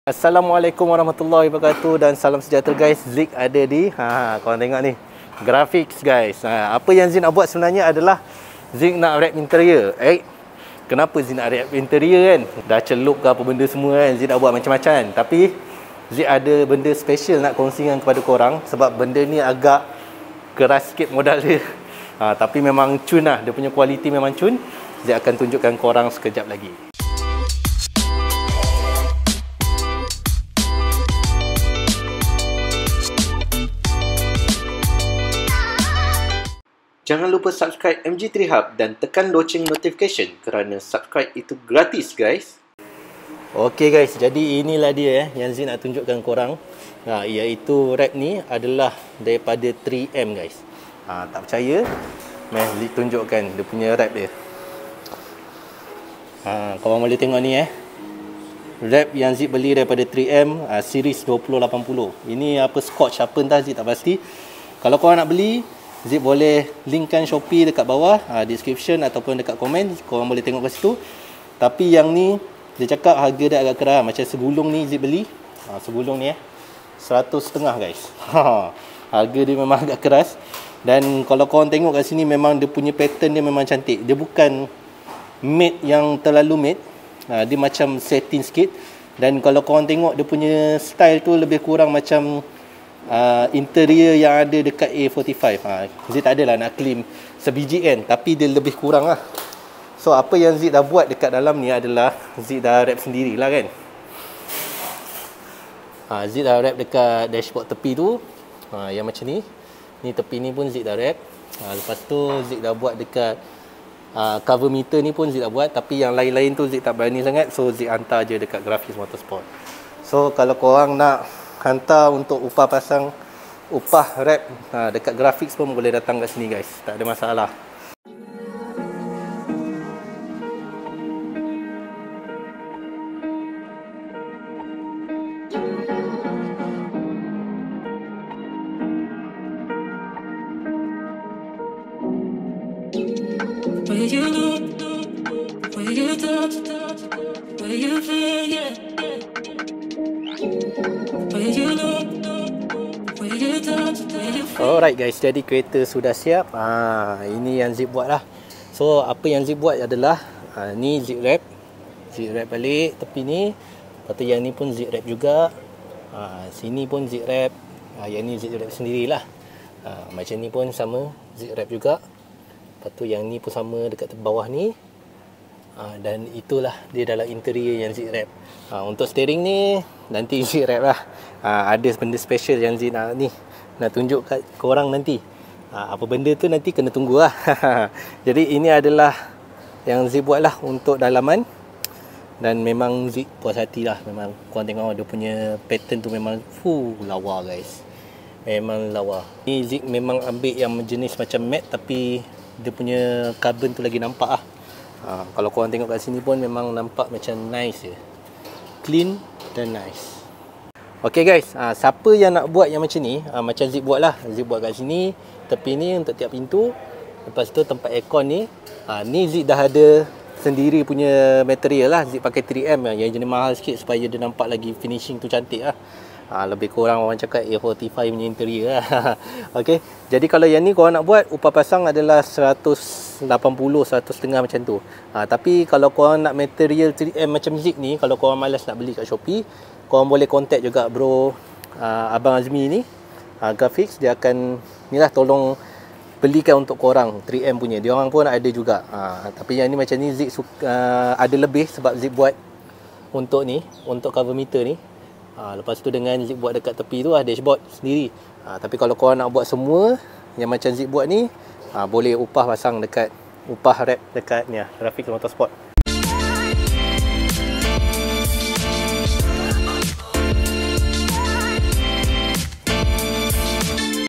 Wrap CARBON FIBER KAYANGAN BRAND 3M!
Ziq sengaja nak beli wrap carbon fiber yang mahal ni tengok rupa dia macam mana. Rupanya ya ampun, memang lawa sampai ziq teracun nak wrap satu interior guna carbon fiber tu. Texture dia betul-betul premium. Saksikan video ini!